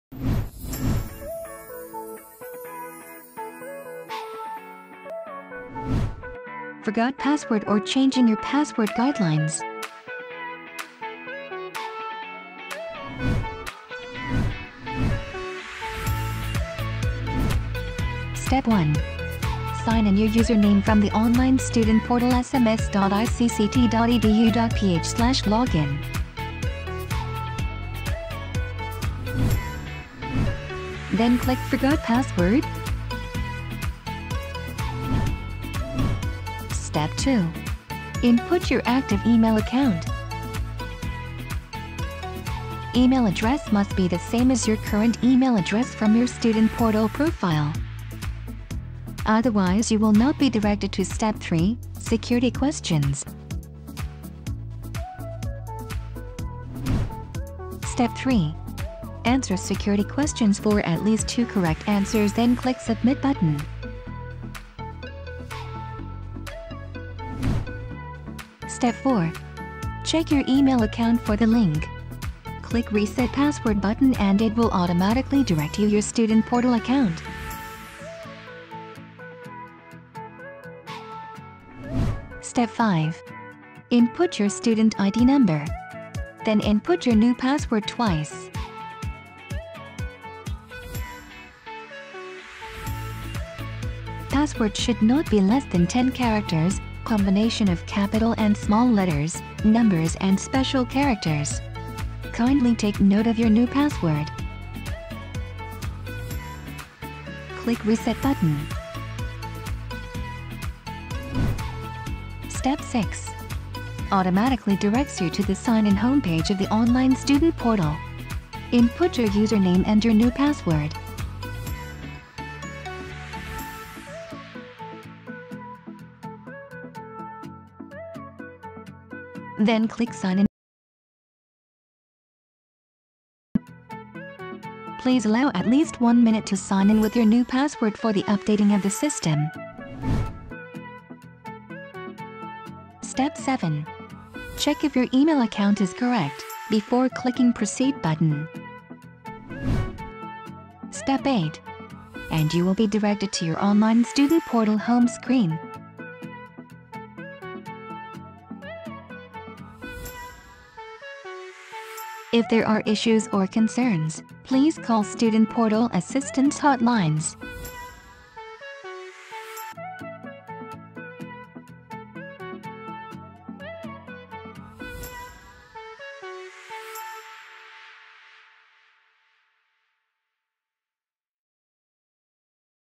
How to use FORGOT PASSWORD feature of your ICCT Student Portal account?
Learn how to recover your account in ICCT Student Portal by using the FORGOT PASSWORD function.